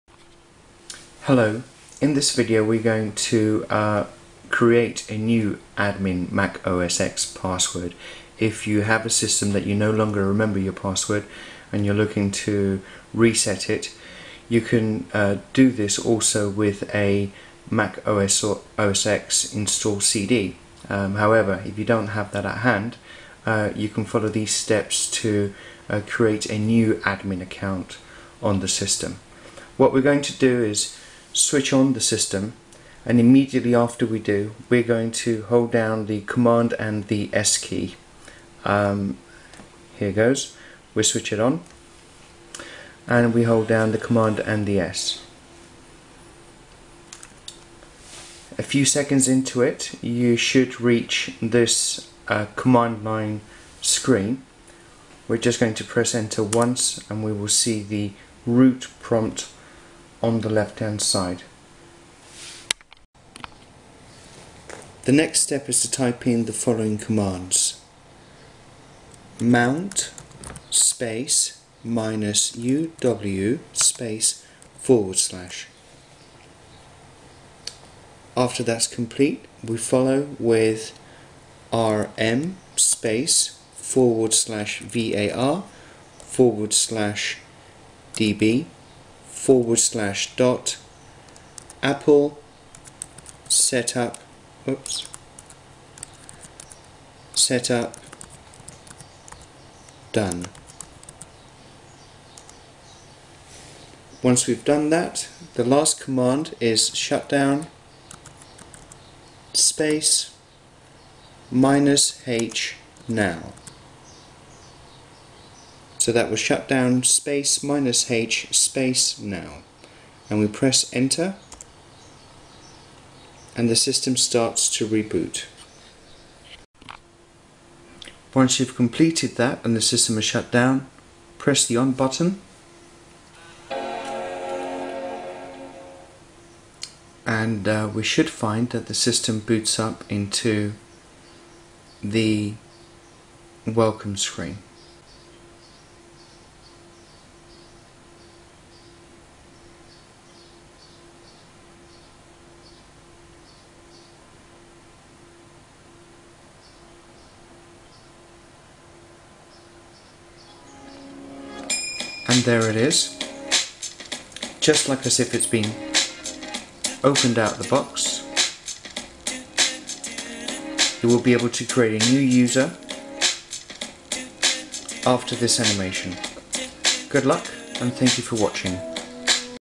Hack remove reset admin Mac OSx password without cd
So your in front of your Mac, its asking for a password and you don't know it.

Can you still get in? Yes you can, heres how.

The first option, if you have access to you system disk then with that cd you can reset the password by booting from it (to boot from cd press the C key on startup)

However if you don't have the cd

The process below will allow you to create a new admin account and with that new account you will be able to login into your system.

You don't loose your files, but you must make a unique username and NOT the same as what originally existed.

Before you start this is what you should know about this procedure.

What your about to do is delete a folder called AppleSetupDone.

By removing this folder it will fool the system into thinking it has not yet completed the setup of creating a new user, so after the reset it will start asking you for your details, just like as if its the first time its running.

Will you loose you files? To avoid confict you should create a unique account and not one of the same name as the system had previously.

Once the new account has been created to get to the documents you have to goto the users folder and change permissions of the folder to get acess your previous documents.

Ive only tested this using Mac OSX, i would assume it would work on any hardware thats running OSX.

Here goes, first you need to enter terminal mode

1. Reboot
2. Hold apple key (or command) + s key down after you hear the chime.
3. When you get text prompt enter in these terminal commands to create a brand new admin account (hitting return after each line)

mount -uw /
rm /var/db/.AppleSetupDone
shutdown -h now

4. After rebooting you should have a brand new admin account.

As not to loose files the new admin account must be a new name and not one previously used. I've only tested this on osX.

Once you are logged in, you might want to get access to your old admin accounts file.

I'll be making a Youtube video that shows you all the steps in action